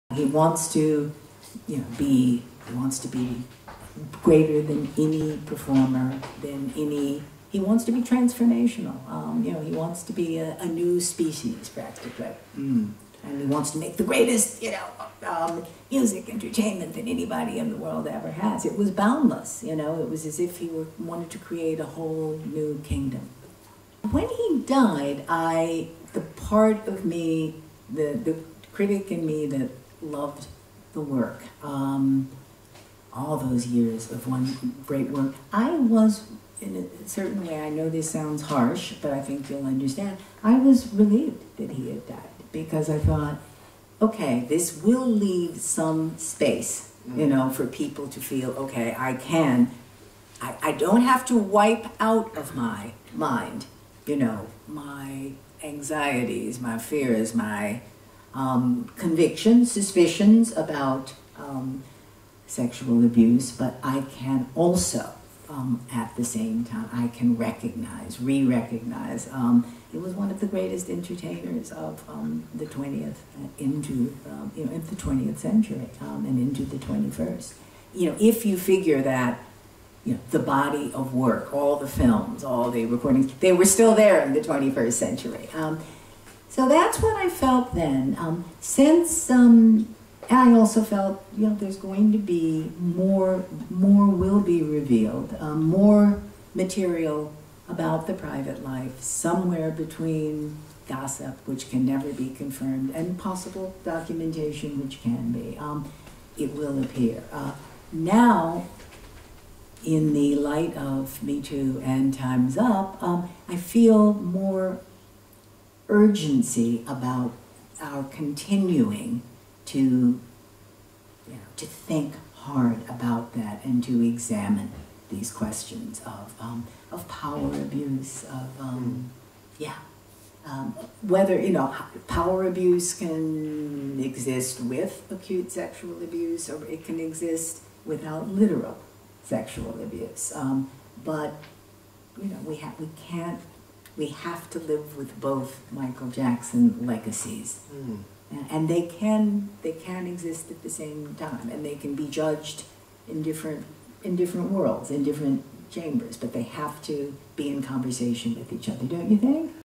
Festival of Ideas: Margo Jefferson on Michael Jackson's legacy
Winner of a Pulitzer Prize and shortlisted for the 2016 Baillie Gifford Prize for Non-Fiction, Margo Jefferson has long been hailed for her ground-breaking writing on African-American culture. Now she explores the many guises of one of the great icons of the 20th century: Michael Jackson. She talks about his legacy.

Filmed by Summer Paliunyte.